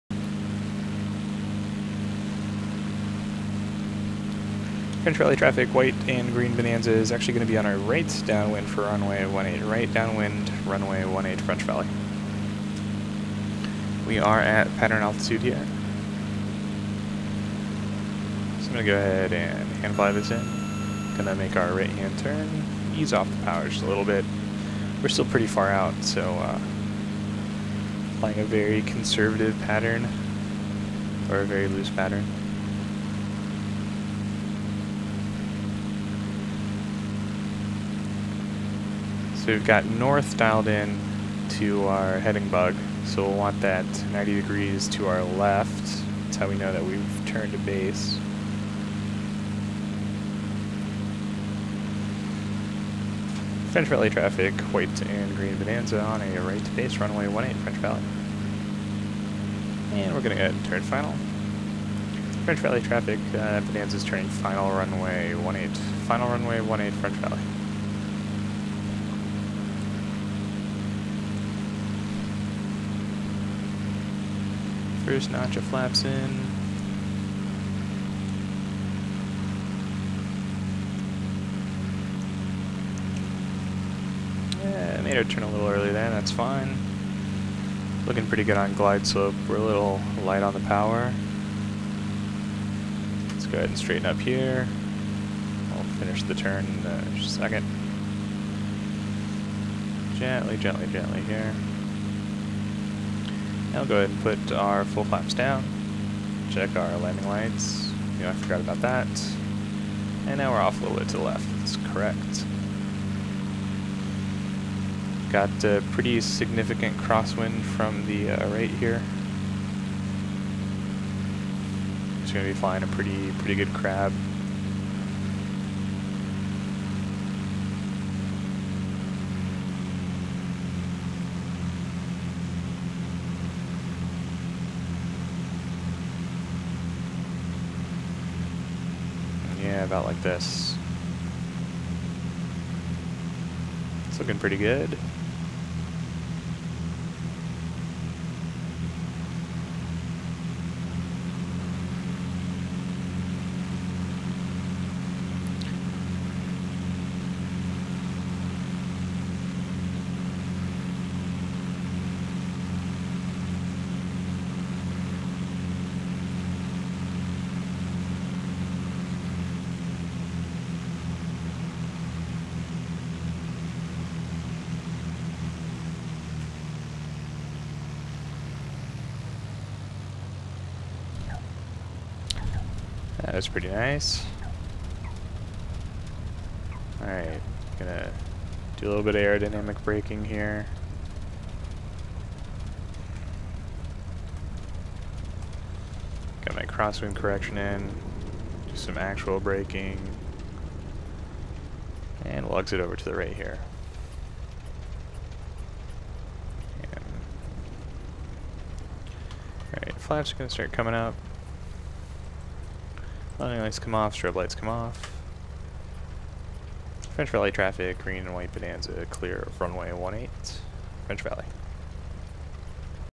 F70 arrival // Bonanza V35 on PilotEdge
Arriving at French Valley Airport (F70) in California.